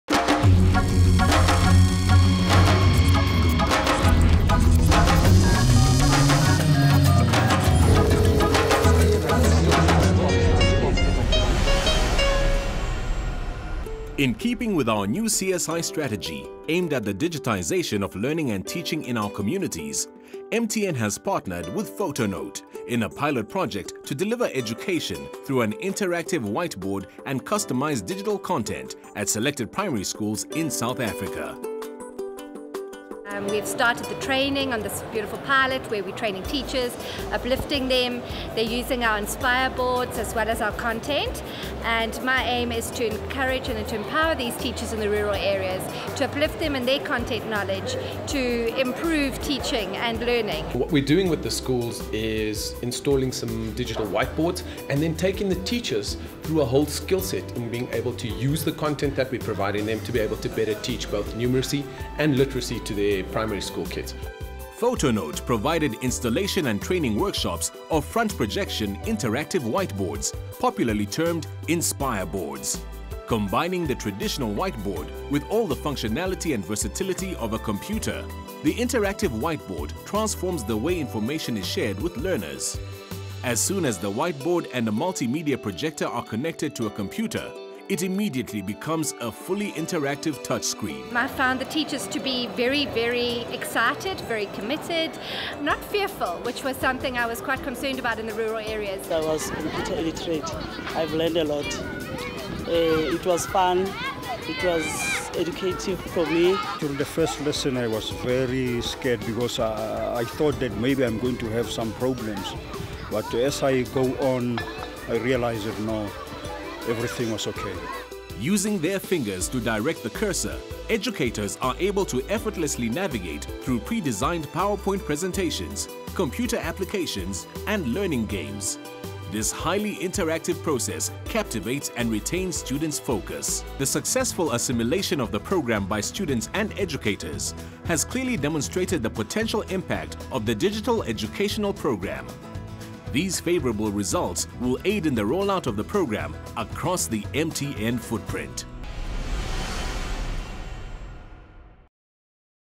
The Digitisation of Education in South Africa
In keeping with our new CSI strategy aimed at the digitisation of learning in our communities, MTN South Africa partnered with PhotoNote, to deliver education through an interactive white-board and customised digital content at selected primary schools in South Africa.